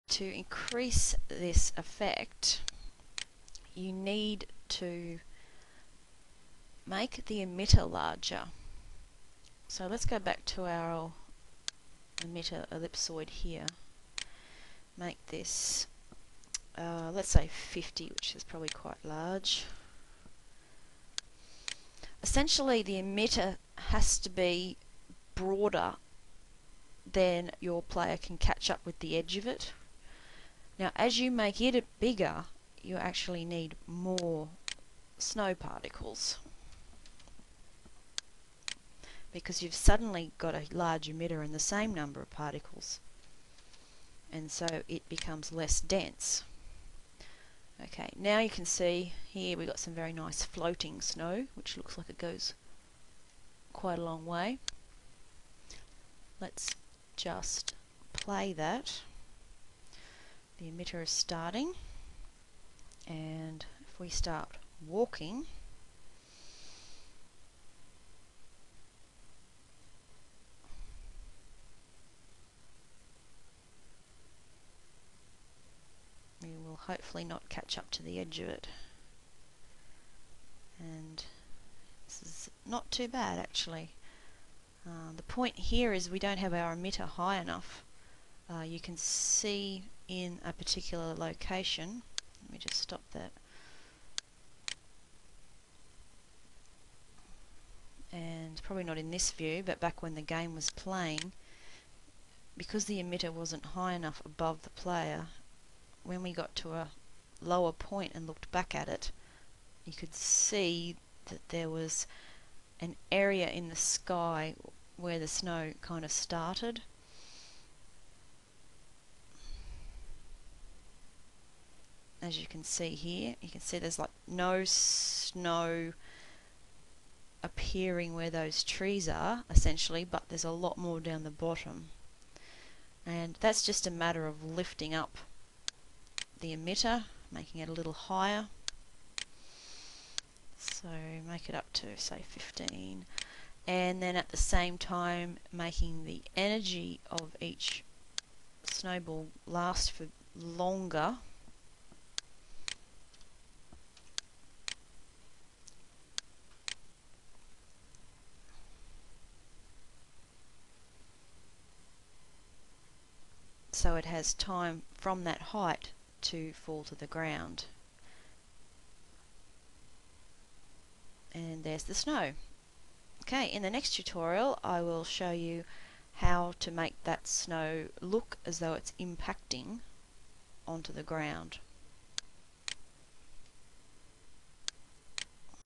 Unity3D: How to Make Snow Part 2
A tutorial in four parts which shows you how to make snow in Unity3d.  You'll need to watch them all in order for it to make complete sense.

A particle system for snow is first created followed by snowball collisions which produce splashes of snow on the terrain surface.